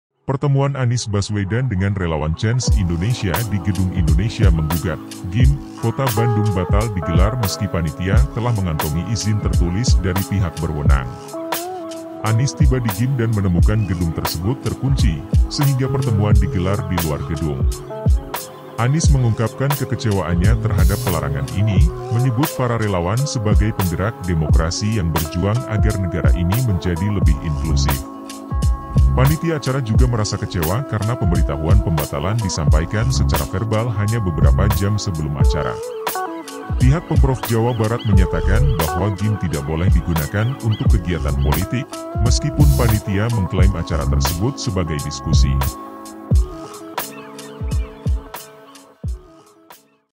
GEDUNG DIGEMBOK, ANIES DAN RELAWAN LESEHAN DI LUAR GEDUNG
Pertemuan Anies Baswedan dengan relawan Change Indonesia di Gedung Indonesia Menggugat (GIM) Kota Bandung batal digelar meskipun panitia telah mengantongi izin tertulis dari pihak berwenang. Anies tiba di GIM dan menemukan gedung tersebut terkunci, sehingga pertemuan digelar di luar gedung. Anies mengungkapkan kekecewaannya terhadap pelarangan ini, menyebut para relawan sebagai penggerak demokrasi yang berjuang agar negara ini menjadi lebih inklusif. Panitia acara juga merasa kecewa karena pemberitahuan pembatalan disampaikan secara verbal hanya beberapa jam sebelum acara. Ketua BaleAmin Jabar, Dr. KH Maman Imanulhaq, menyebutnya sebagai preseden buruk dalam tata kelola acara di ruang publik yang dimiliki pemerintah. Pihak Pemprov Jawa Barat menyatakan bahwa GIM tidak boleh digunakan untuk kegiatan politik, meskipun panitia mengklaim acara tersebut sebagai diskusi.